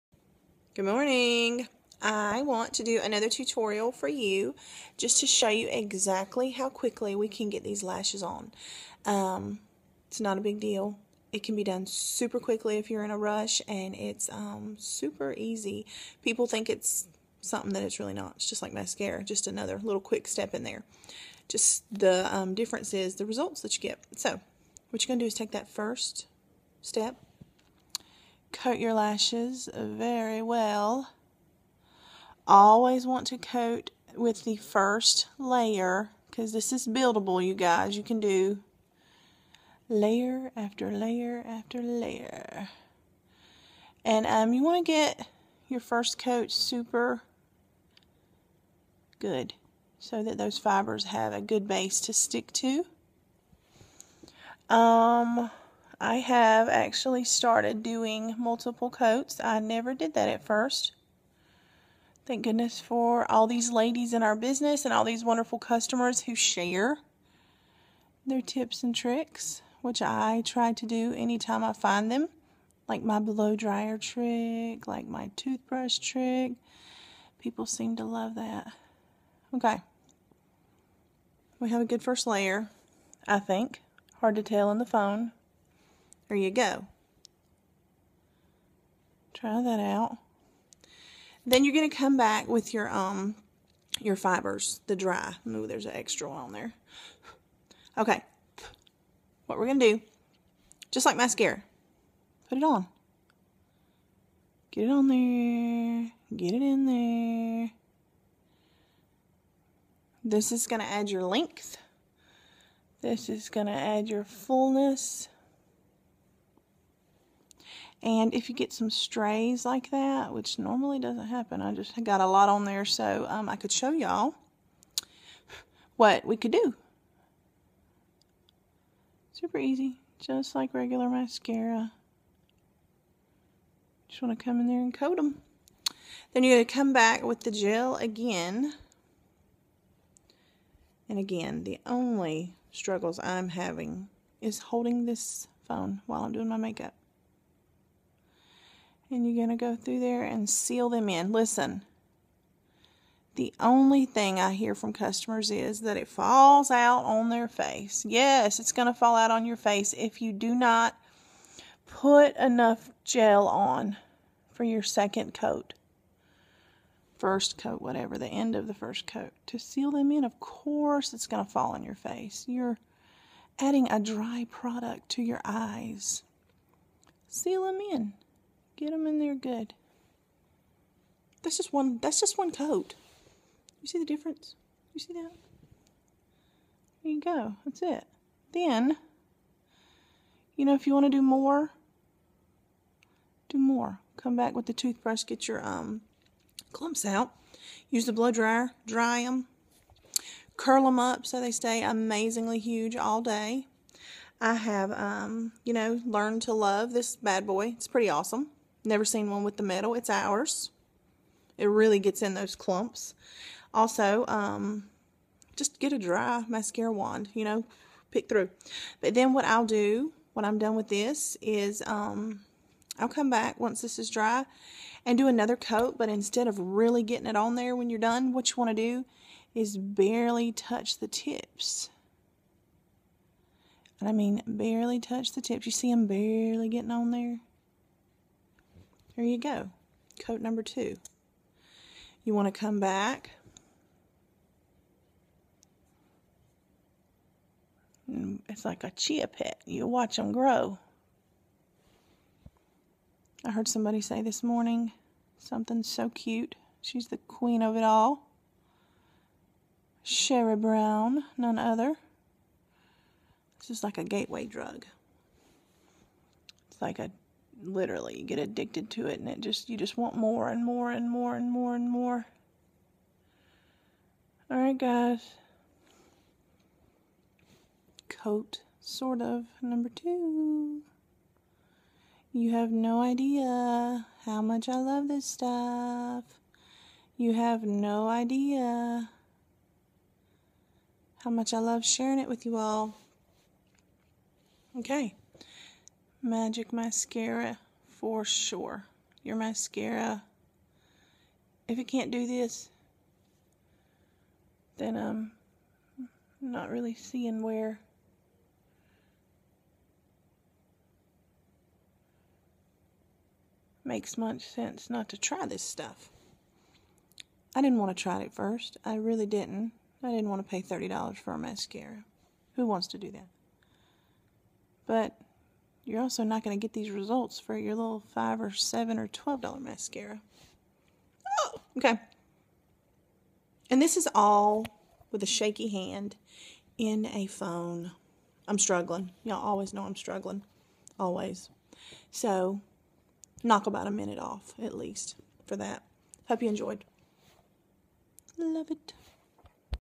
Younique's 3D Fiber Mascara EASY application
I wanted to redo my application video to highlight some pointers and show how quickly this can be done! Get your 3-D mascara here and if you have any questions as always I am more than happy to help!